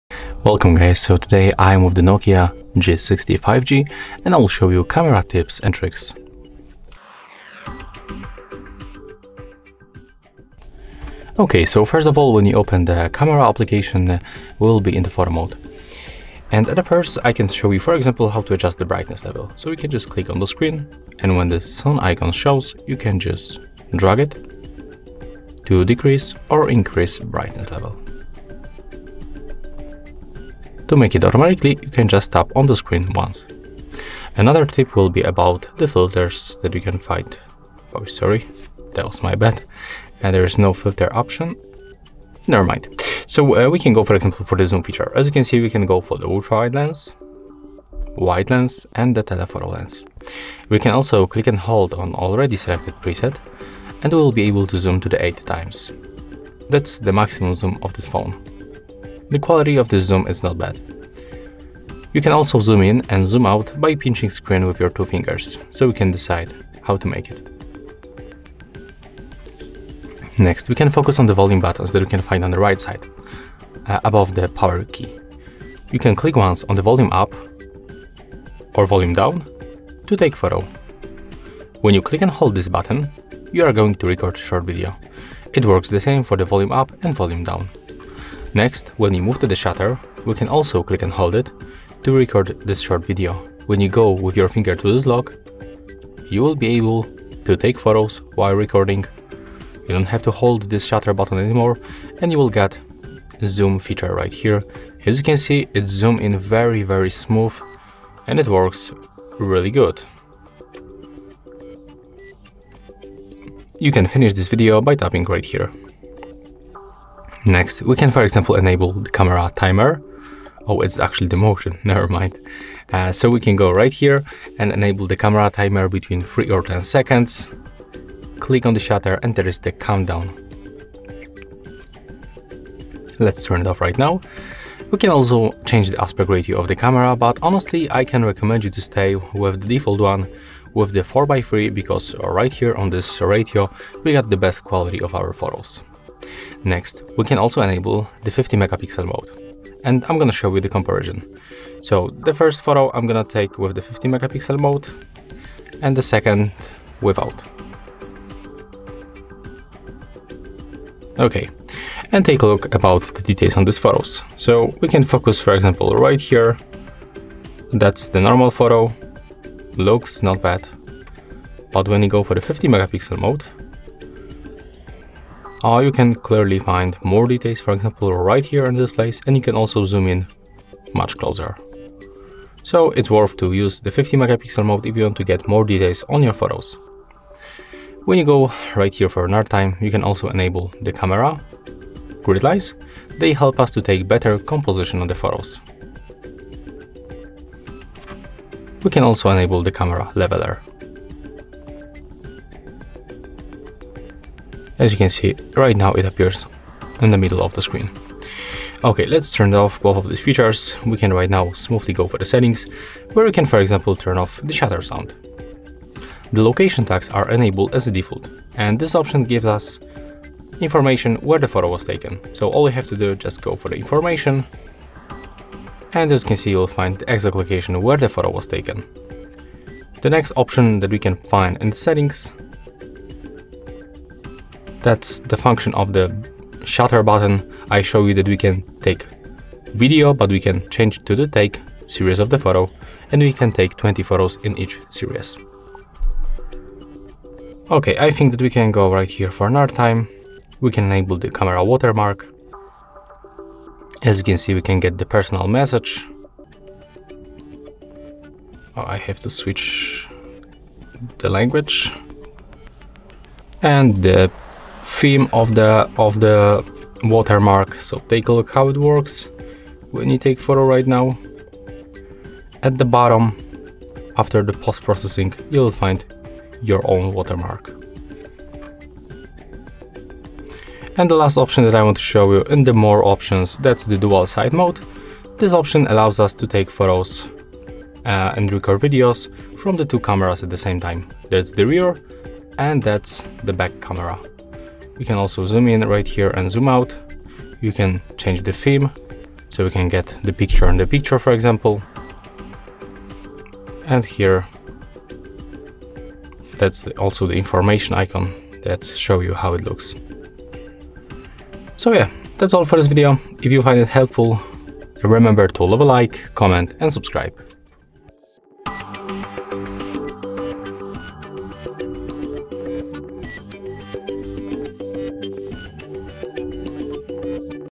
NOKIA G60 Camera Tips Tricks
Find out more info about NOKIA G60 5G:
If you want to know the best camera tricks in NOKIA G60, check out this tutorial. Our expert will show you some interesting modes and functions that make the photos very clear, not overexposed and perfectly framed. Go to our YouTube channel to learn more about NOKIA G60.

How to use top camera tricks on NOKIA G60? How to use the best features on NOKIA G60?